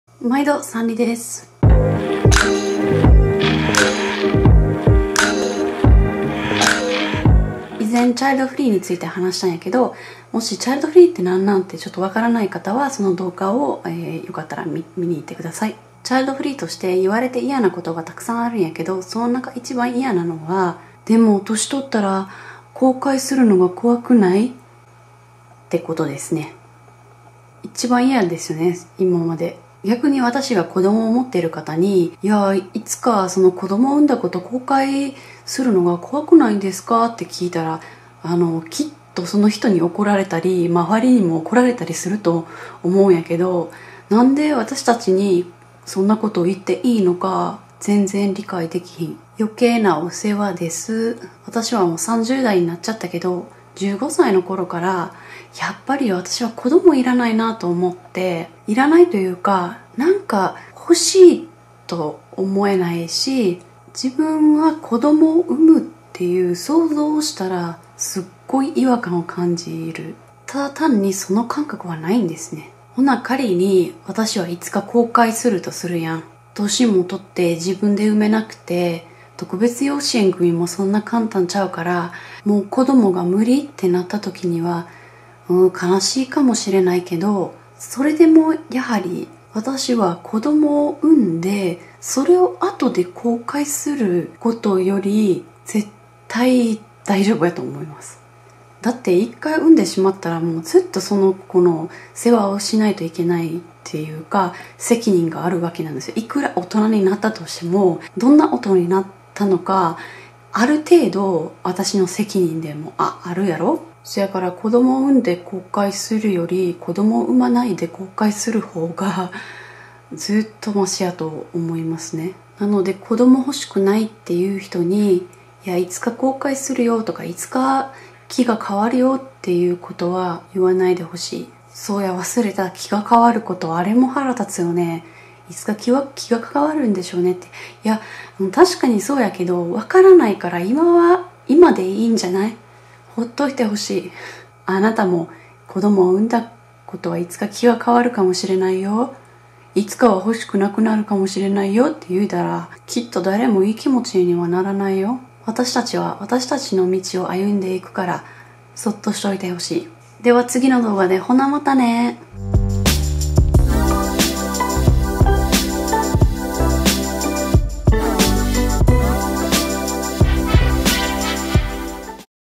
【チャイルドフリー】言われて一番嫌なこと
こんなこと言うのをやめましょうね。
自分が子供を産んでよかったと思って、そう思えない人もいます。
私は、自分で決めます。